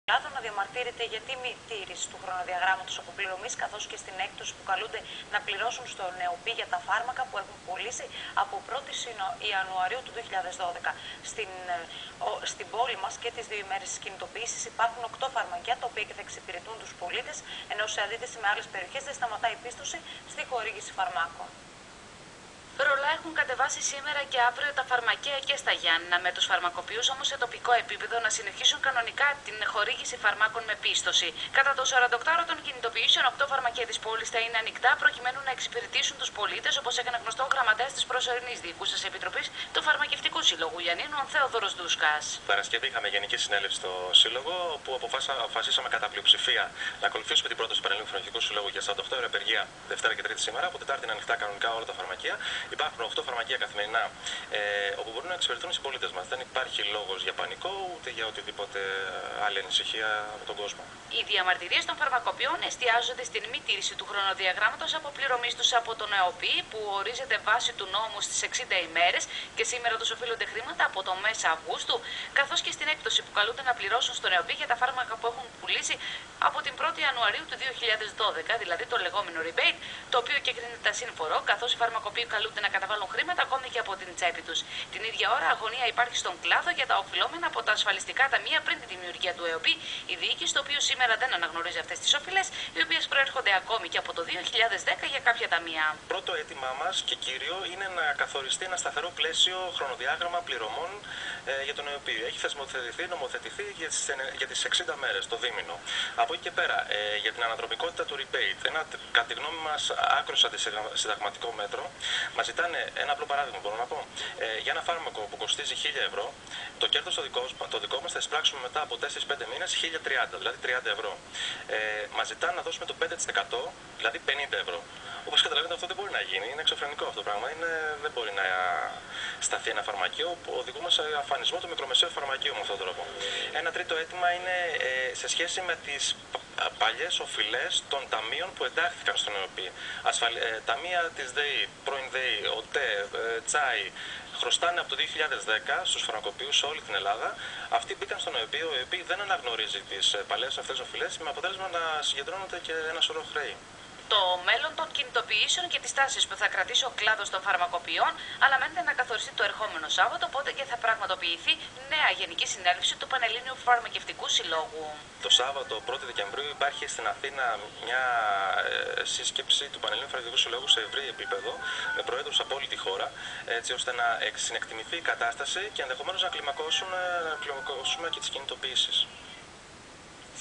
farmakopoioi: Κινητοποιήσεις και στα Γιάννενα
Ο γραμματέας της προσωρινής διοικούσας επιτροπής Θεόδωρος Ντούσκας μιλά για τις κινητοποιήσεις των φαρμακοποιών στα Γιάννενα.